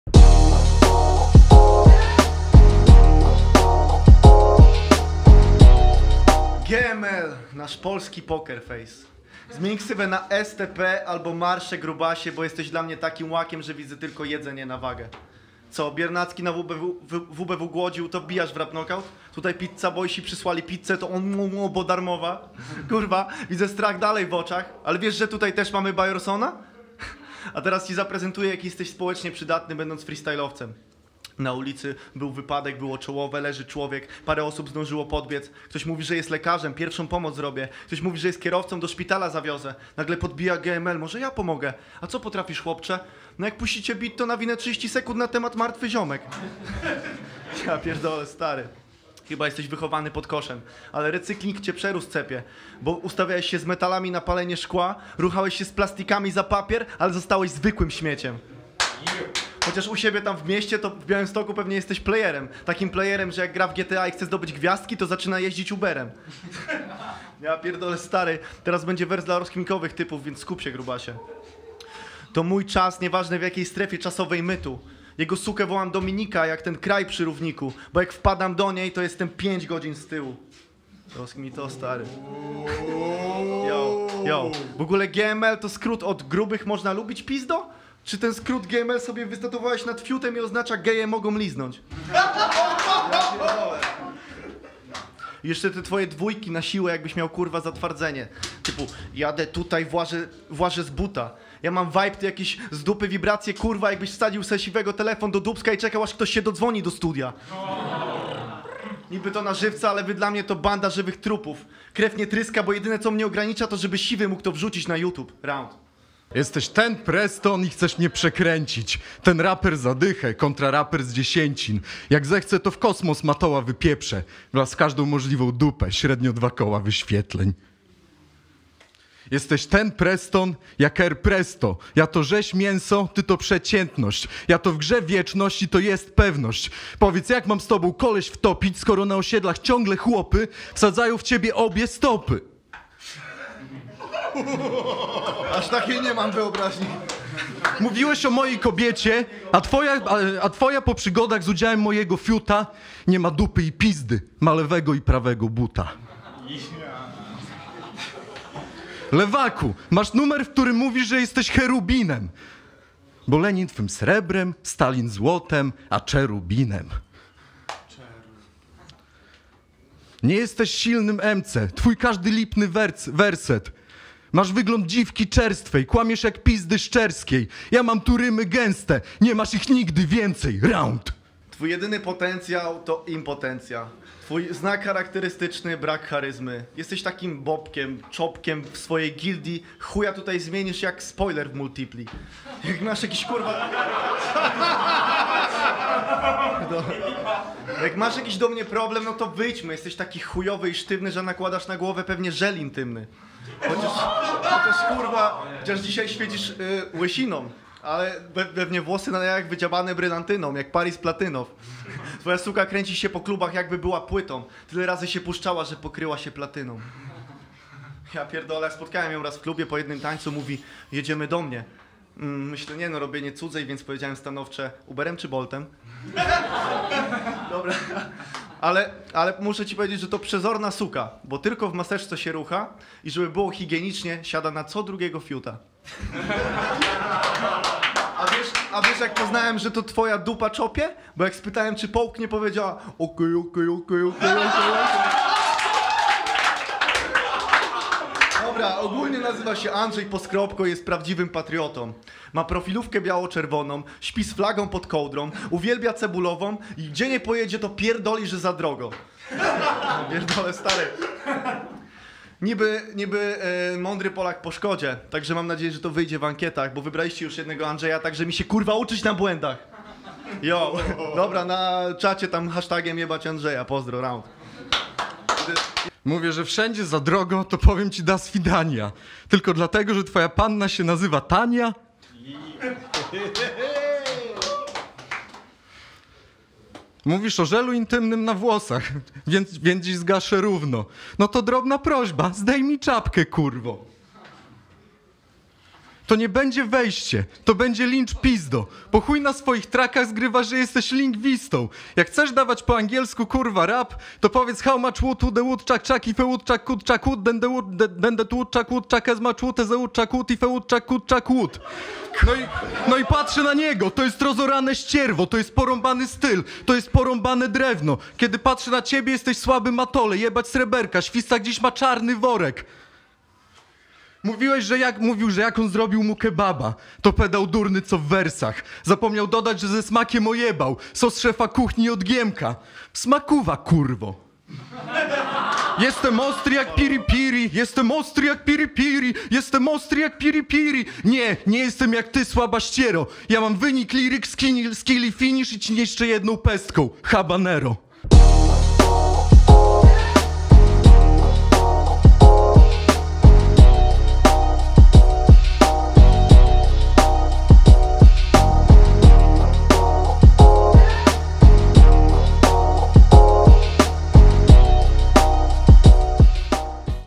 Ten Preston vs. GML ⚡️ RN4 ELIMKI | KTO WYGRAŁ? ZAGŁOSUJ!
Oglądaj turniej Rapnokaut 4 Patriotic Stage Cup!

Kto według CIEBIE wygrał tę walkę? TEN PRESTON CZY GML?

Jest to walka z GRUPY A, do której należą:
ŁUUKI
TEN PRESTON
YOU_REC
GML

Spośród 16 zawodników, którzy brali udział w eliminacjach, 6ciu z nich zawalczy na Rapnokaut 4!

CZWÓRKA MC's zostanie wyłoniona przez głosowanie! Możesz mieć na to wpływ!
POD KAŻDĄ WALKĄ jest LINK DO ANKIETY, który przekieruje Cię na RAPNEWS.PL i ... wystarczy, że ZAGŁOSUJESZ!

Pozostała DWÓJKA, otrzyma DZIKĄ KARTĘ od organizatorów RAPNOKAUT i ekipy RAPNEWS.PL

OBSERWUJ NAS, BO NIEDŁUGO ZACZNIEMY ODSŁANIAĆ KARTY!

Prawdziwe newsy o polskim rapie.